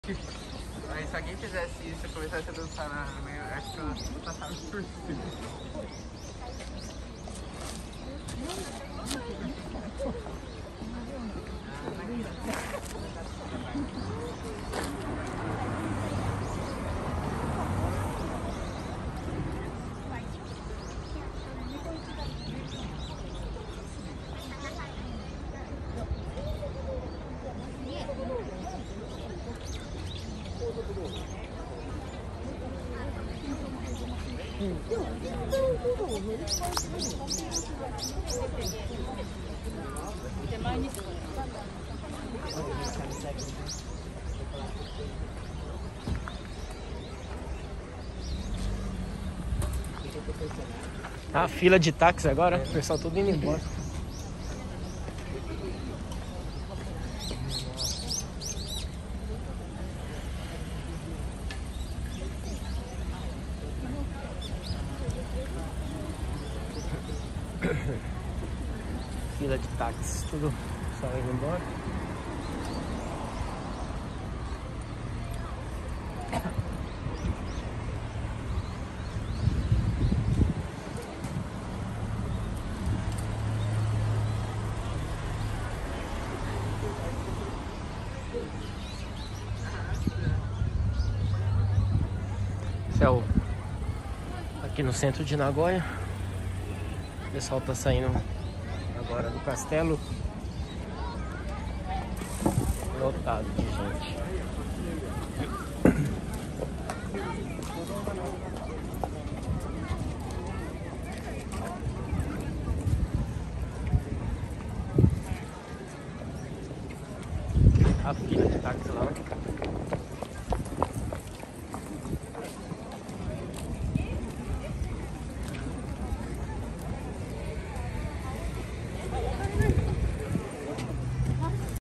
ADMIRANDO OS SAKURAS E O CANTO DOS PÁSSAROS NO CENTRO DE NAGOYA JAPÃO CANAL ALECS JAPAN LIFE
ADMIRANDO OS SAKURAS E O CANTO DOS  PÁSSAROS NO CENTRO DE NAGOYA JAPÃO CANAL ALECS JAPAN LIFE@alecsjapanlife